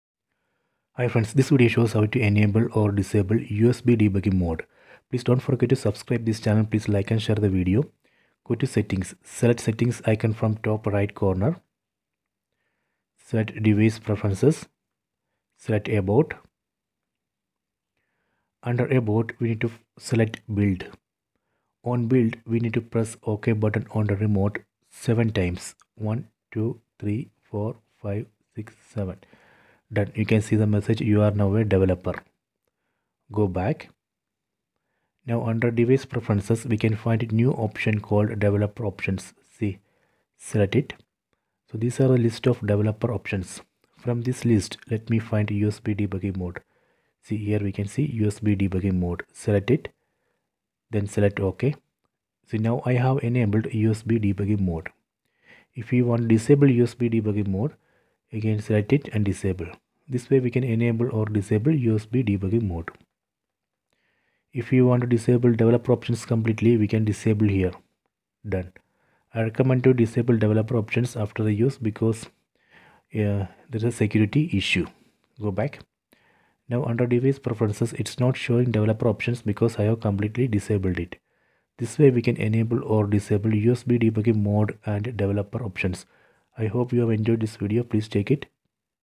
Croma Android TV : How to Enable or Disable USB Debugging Mode
Android TV Google TV : How to Enable or Disable USB Debugging Mode,
Android TV,
Android,
Smart TV,
Smart Google TV,
Google TV,